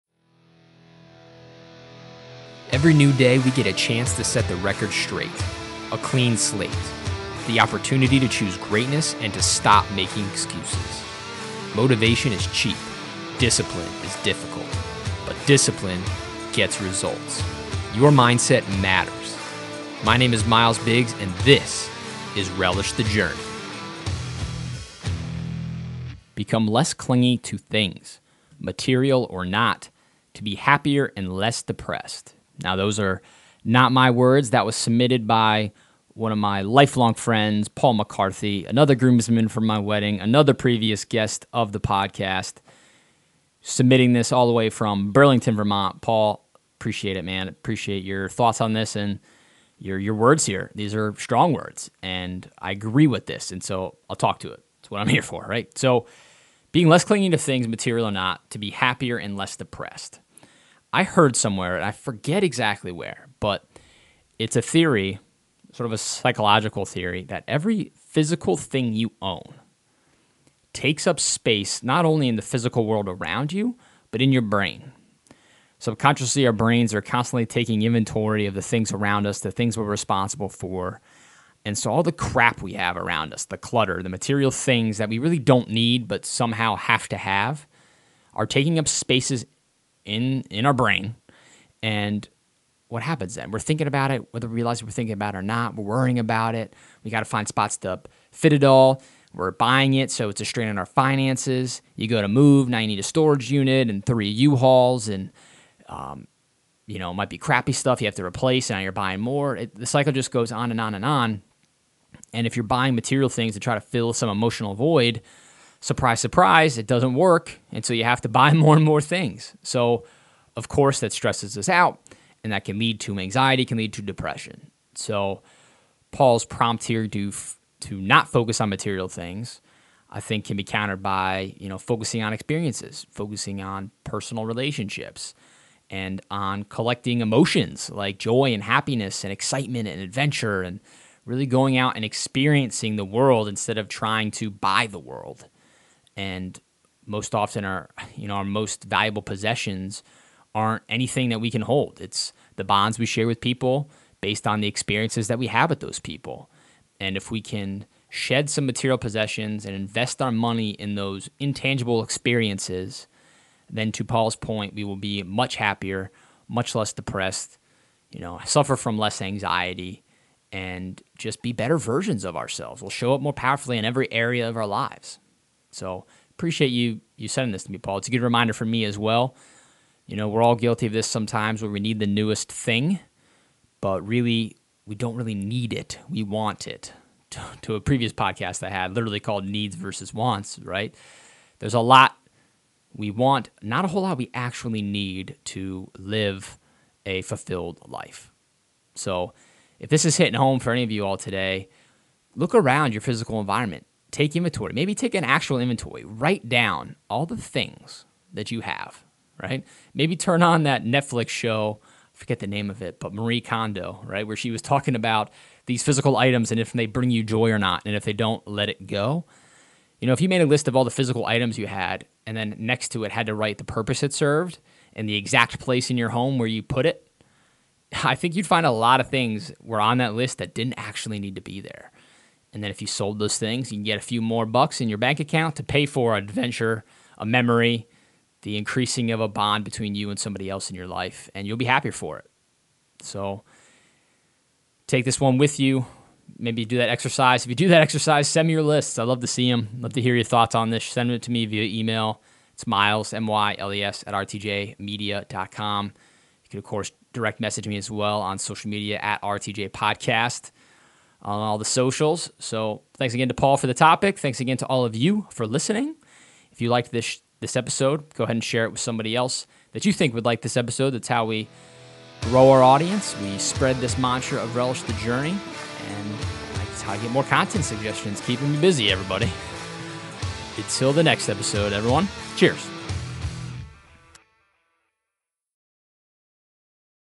Do More (And Be More) With Less | Relish The Journey Podcast
Every physical item you own is taking up space in your mind. Physical clutter = mental clutter and it's up to all of us to make the change.

Special thank you to Paul McCarthy for our content topic today. Hear his interview on Relish The Journey (S1E13)